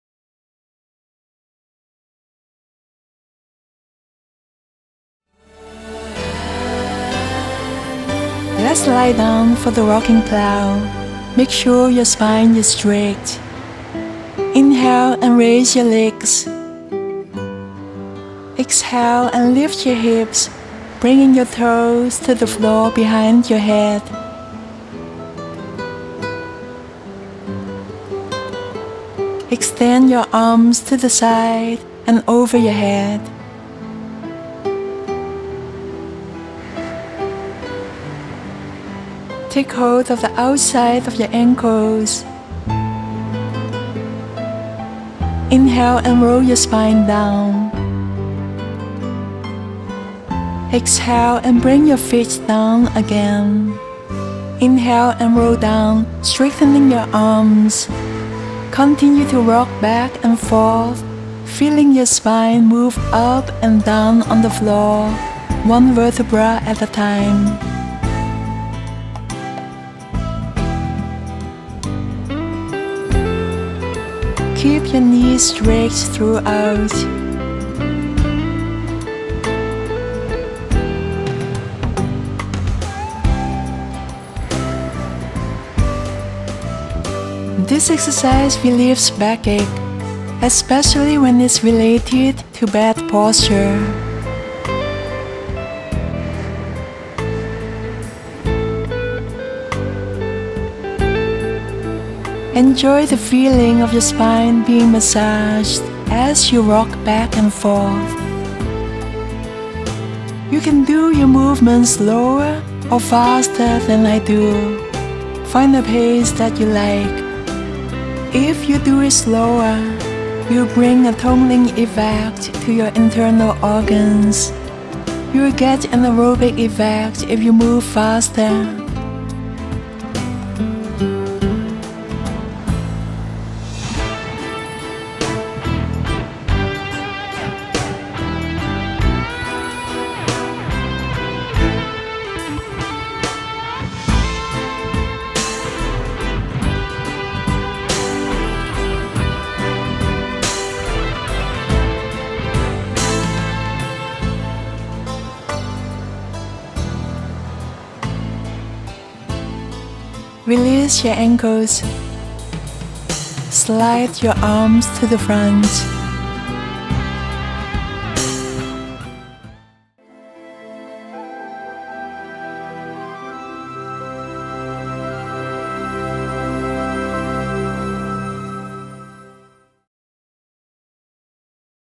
Get Relieved from Backache with Rocking Plough by Wai Lana
Learn Yoga with Wai Lana's Beginner Yoga DVD Series -

Rocking Plough by Wai Lana helps relieve backaches.
It massages the spine and tones the internal organs.